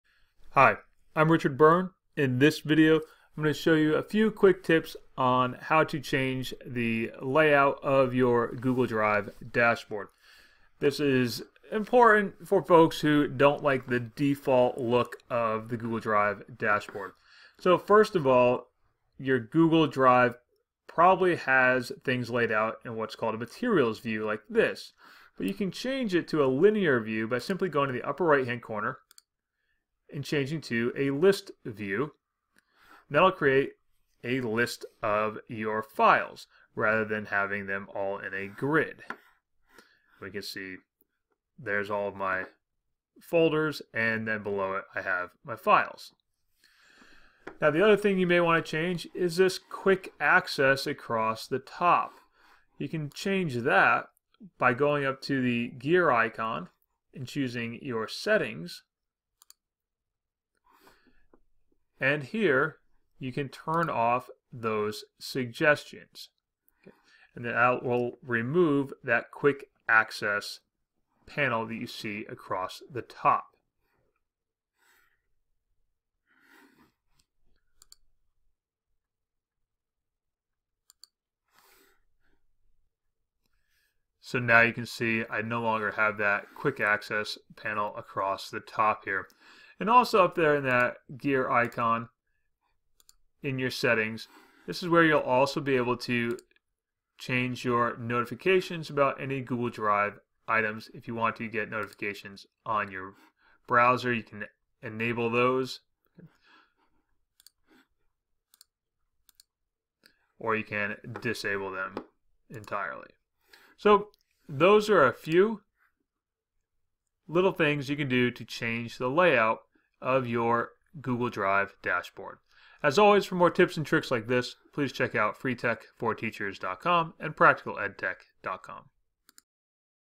Quick Tips for Changing the Layout of Your Google Drive
A couple of quick tips for changing the layout of your Google Drive dashboard.

Learn more about Google Drive in my online course Getting Going With G Suite.